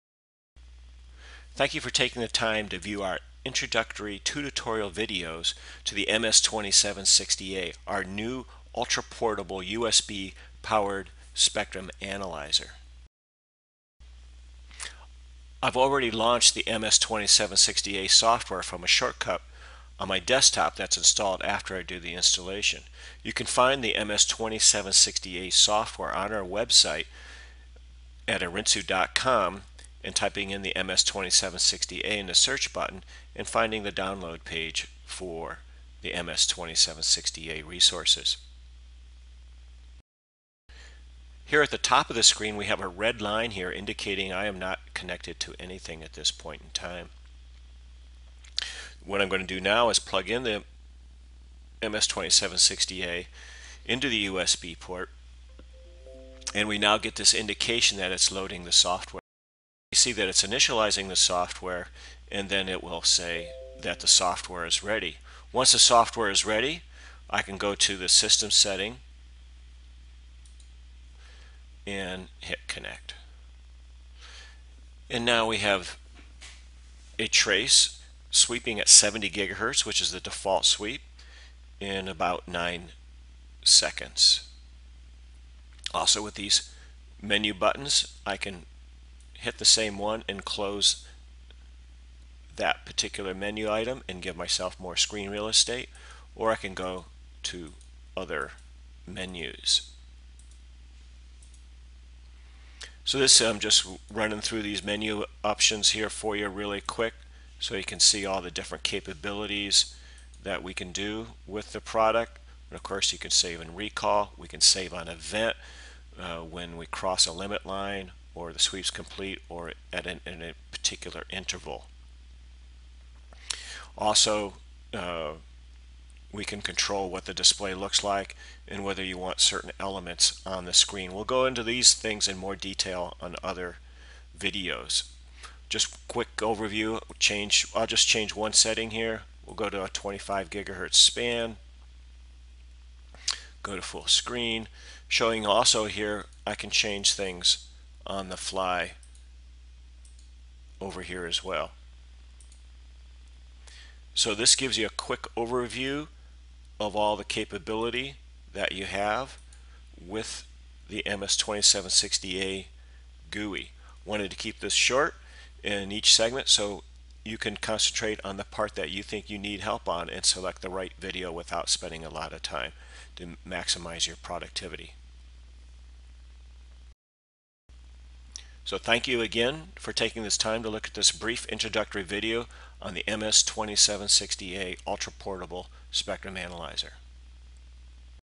Ultraportable Spectrum Master - introducing GUI Overview
This video is part of a series to introduce the features and functions of the MS2760A Ultraportable Spectrum Analyzer.  This video demonstrates a quick overview and introduction of the overall architecture of the user interface.